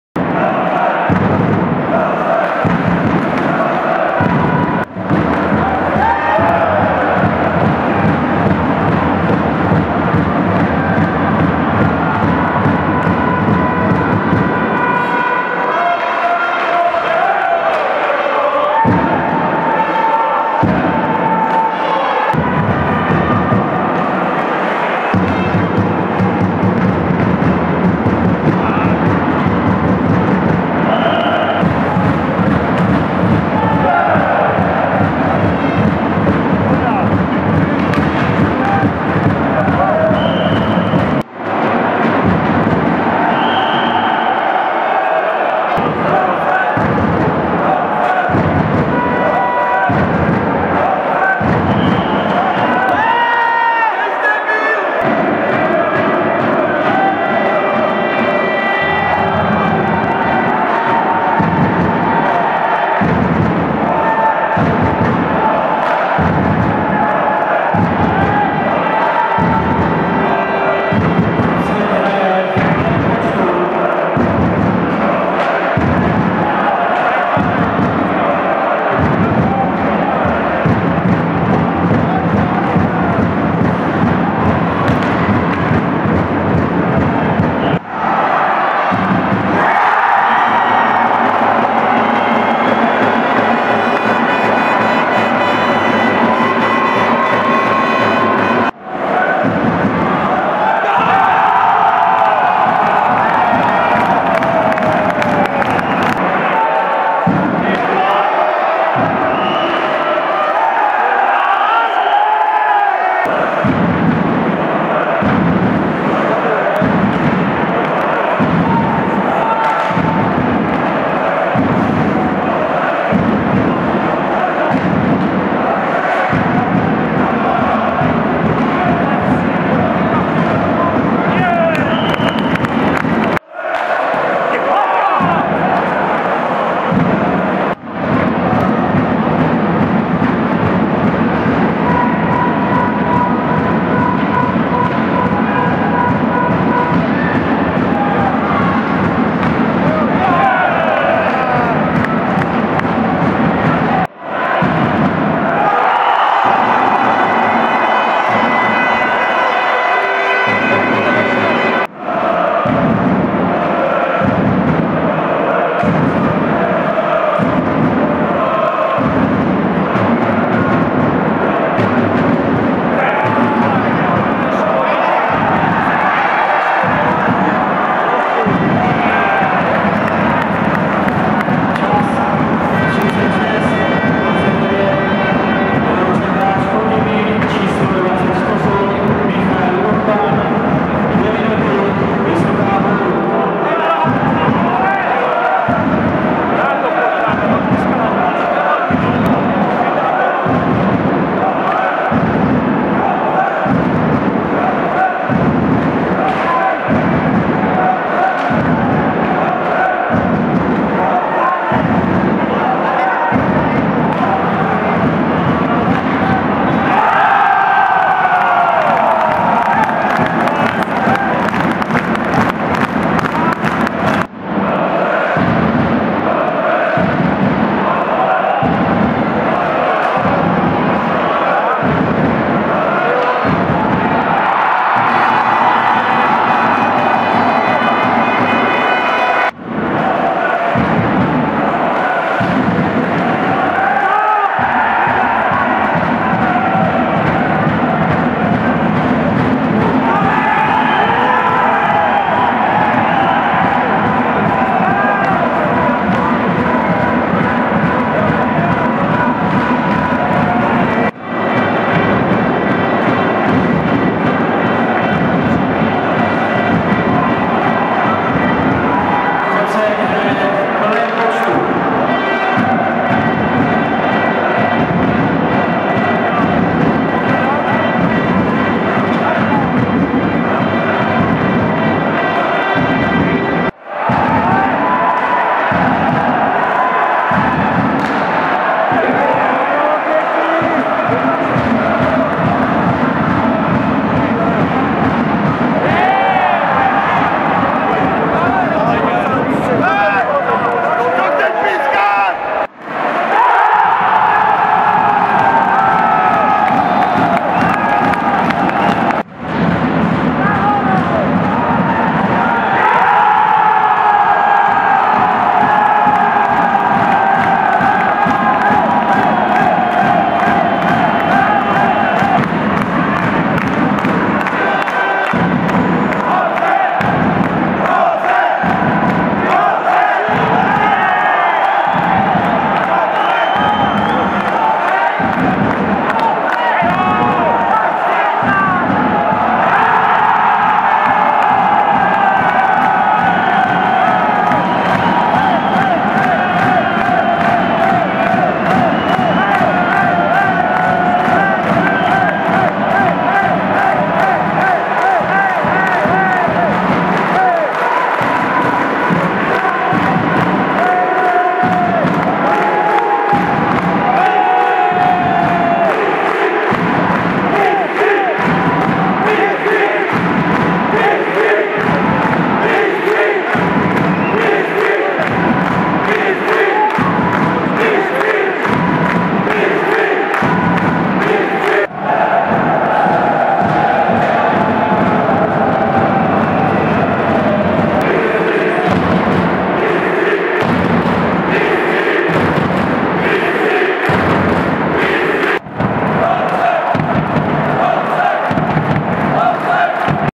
Finále KHL Chrudim vs. Choceň
Ve druhém utkání vyhráli hráči Chocně 6:4 a mohli slavit titul v Krajské hokejové lize. Více informací